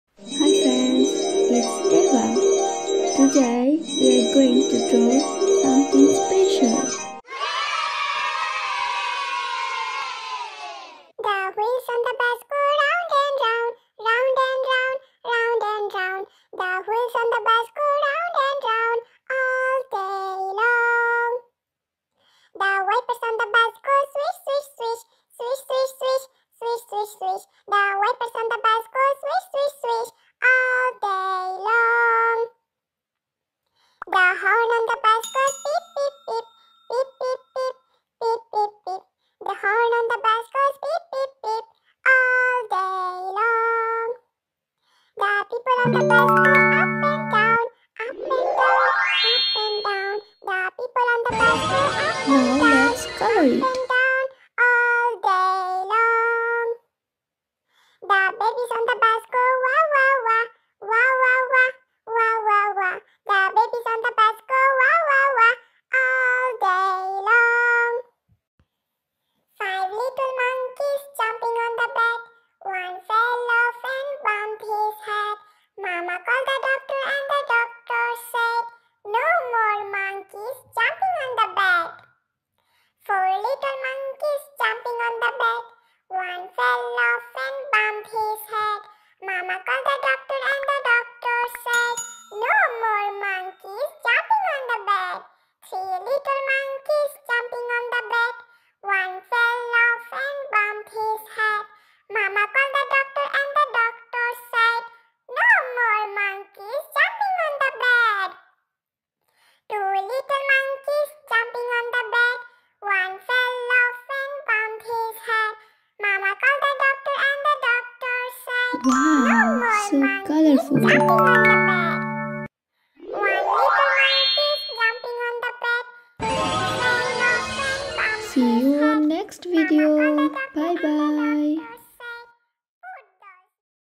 Draw a Cute Jelly Cake | Rainbow Jelly Cake Drawing for Children | Disegnare e Colorare una Torta💛
Hi friends it's Eva.
Welcome to "Eva Art" let's see how to draw and color this super easy jelly rainbow cake drawing tutorial step by step. If you've always wanted easy drawings, here is a tutorial that will make it so much easier!  ♥️💜
Digital hand drawing and coloring.
 ▶ Daily new video.